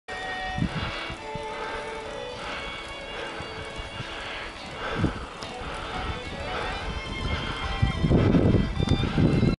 PAC Tour Day 8 - Climbing Oregon Skyline Road
An incredible road with very few cars